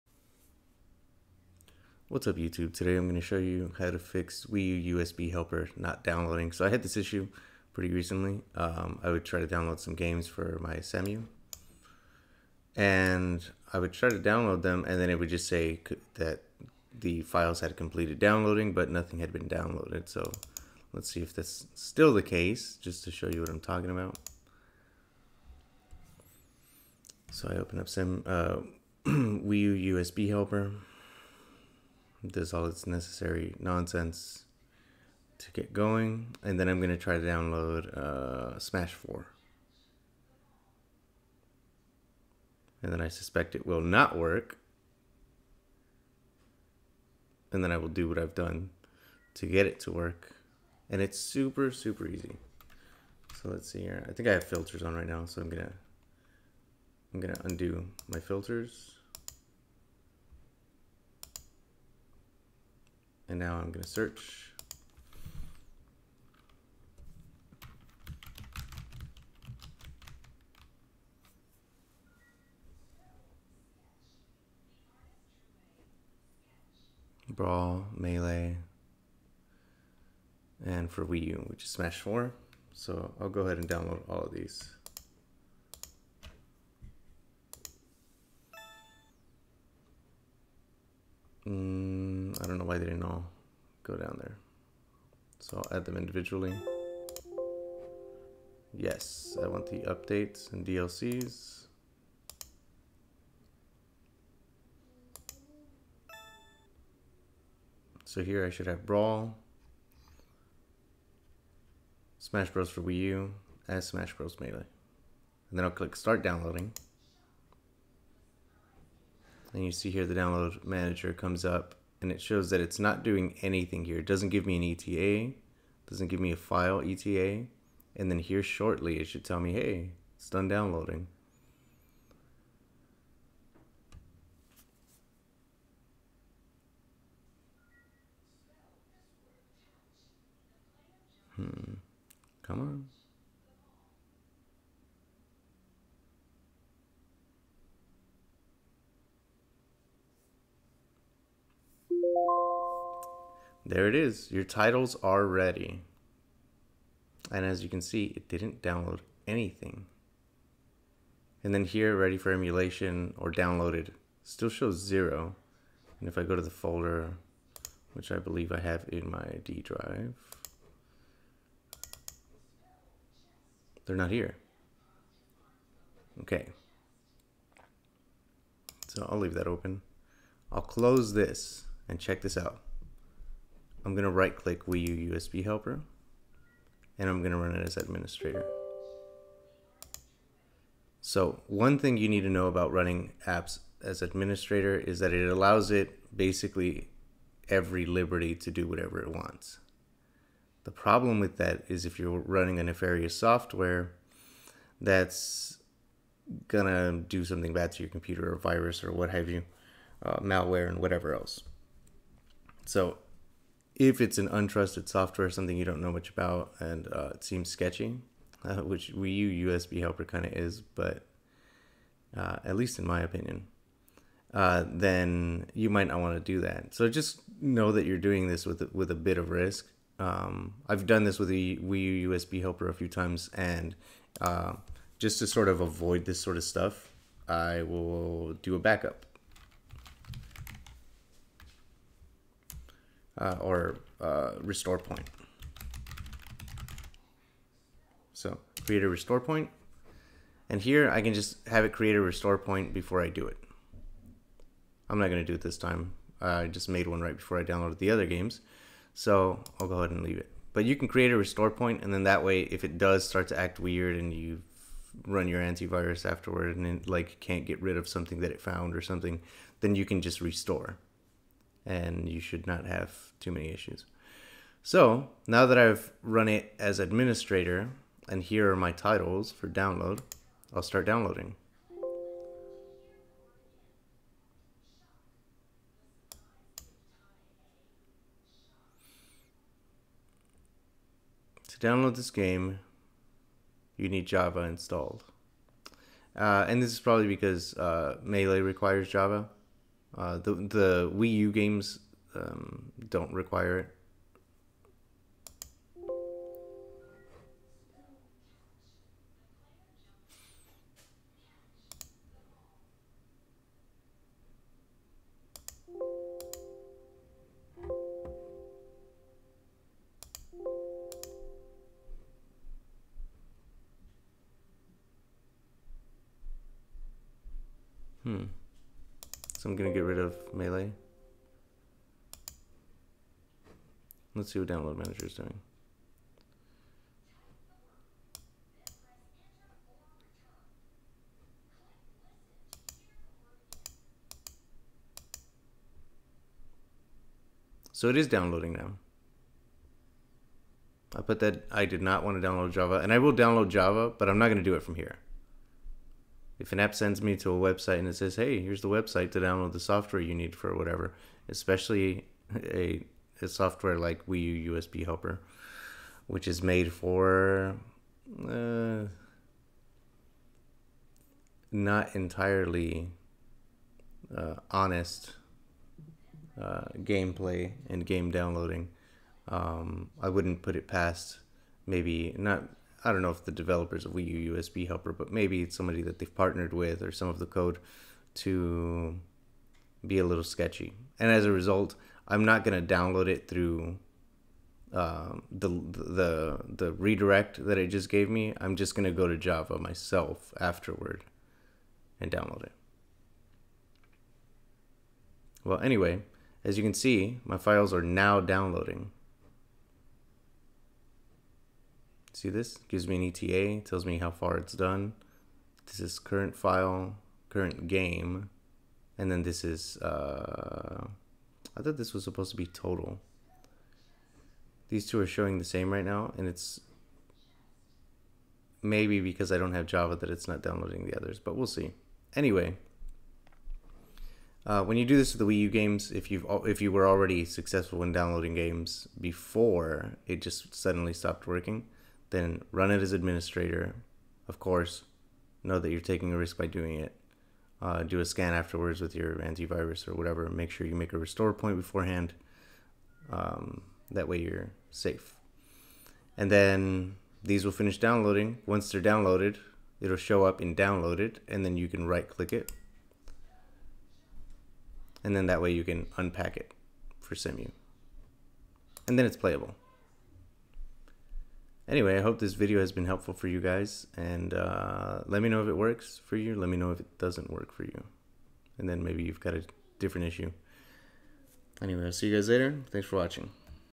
How to fix Wii U USB Helper Not Downloading Games (March 2021)
Make sure you create a restore point before doing this, in case of any malware caused by running it as admin. Another safety measure would be to set compatibility mode in bios to UEFI instead of CSM, and enabling "Secure Boot". At first, I had tried changing the title keys URL, but that didn't work. I found this fix, so I figured I'd share. I hope it helps you.